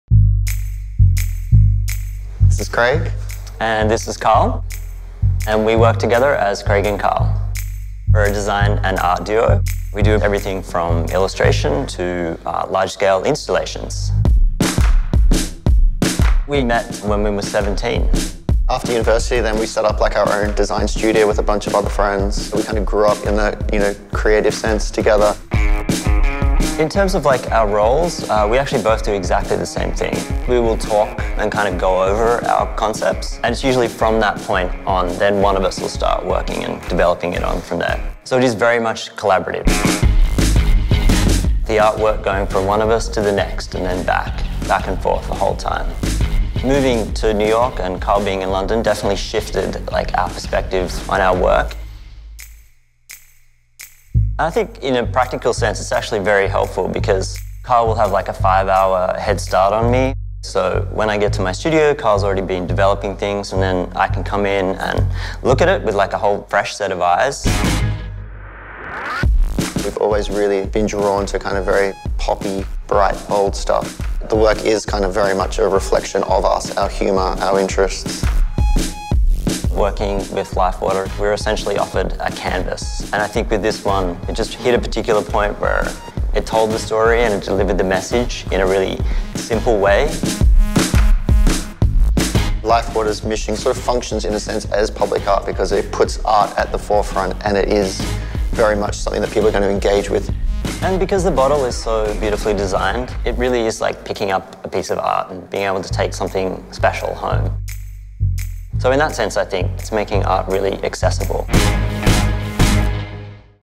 LifeWTR Craig & Karl Vogue
Meet the vision behind LIFEWTR's captivating bottle designs.  World renown artists, Craig & Karl speak to Vogue about their partnership, process and approach to LIFEWTR bottles as their canvas.  Directed by Bon Duke.  Edit and color grading by DE